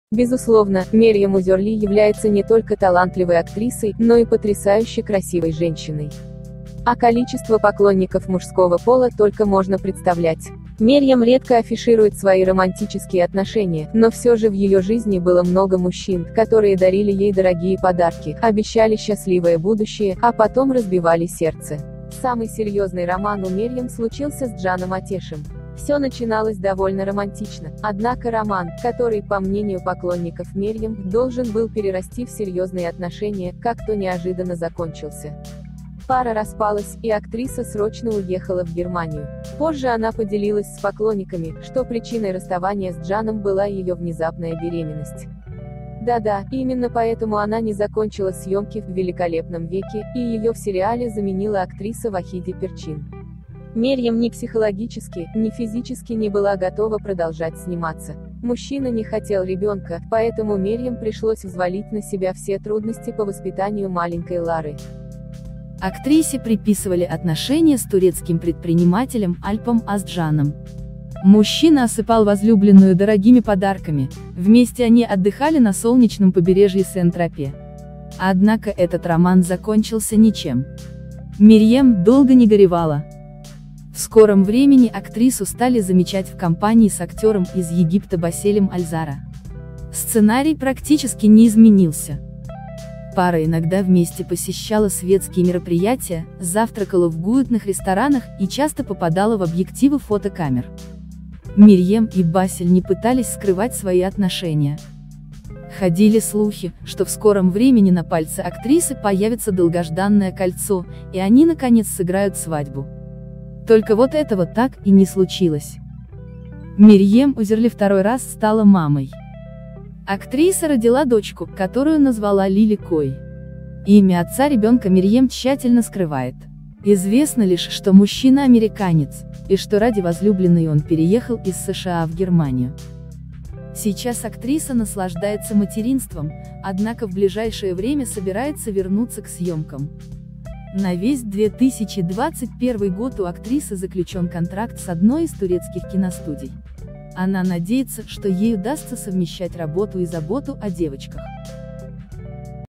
Мужчины актриссы сериала Великолепный век
В этом видео собрана информация о мужчинах в жизни  великолепной актрисы турецких сериалов  Мирьем Узерли. Актрисса несмотря на трудности отношений с мужчинами, смогла обрести женское счастье И стать прекрасной матерью двум девочкам.